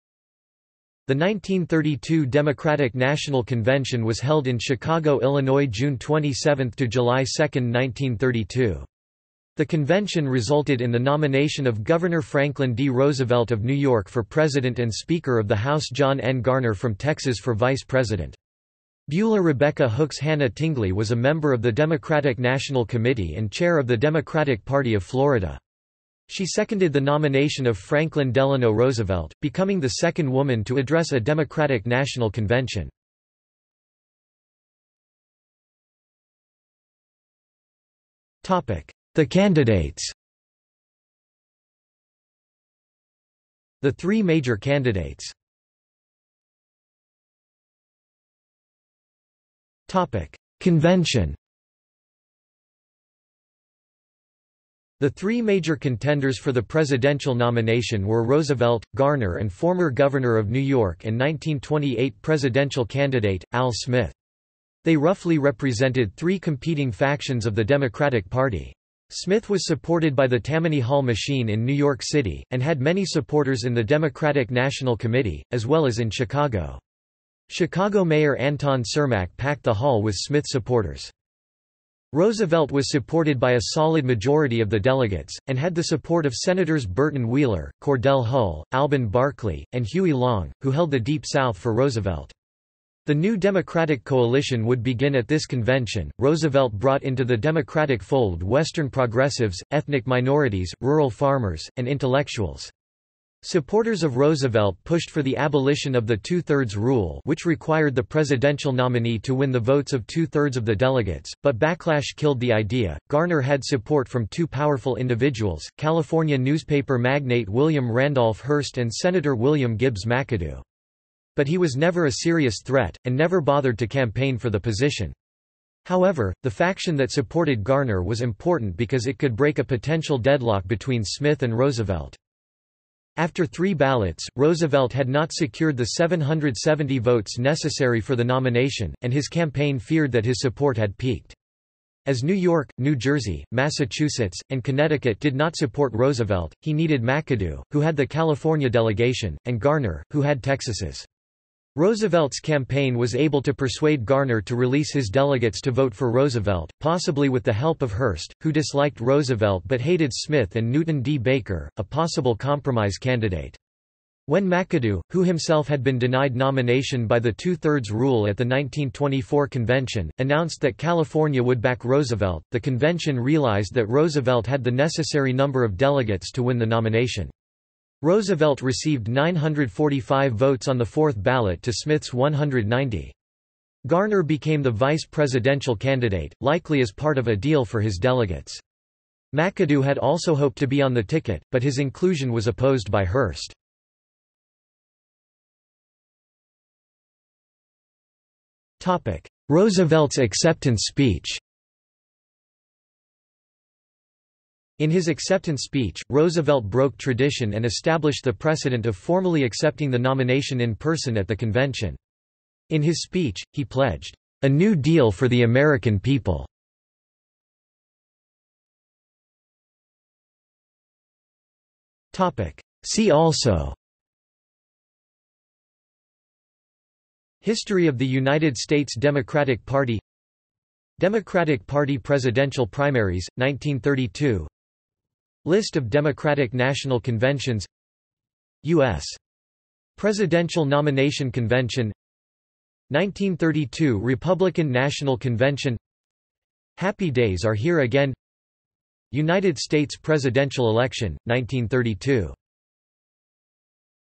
1932 Democratic National Convention | Wikipedia audio article
This is an audio version of the Wikipedia Article:
1932 Democratic National Convention

00:00:34 1 The candidates
00:00:46 2 Convention
00:03:15 2.1 Roosevelt's acceptance speech
00:03:38 3 See also

- Socrates

SUMMARY
The 1932 Democratic National Convention was held in Chicago, Illinois June 27 – July 2, 1932. The convention resulted in the nomination of Governor Franklin D. Roosevelt of New York for President and Speaker of the House John N. Garner from Texas for Vice President. Beulah Rebecca Hooks Hannah Tingley was a member of the Democratic National Committee and Chair of the Democratic Party of Florida. She seconded the nomination of Franklin Delano Roosevelt, becoming the second woman to address a Democratic National Convention.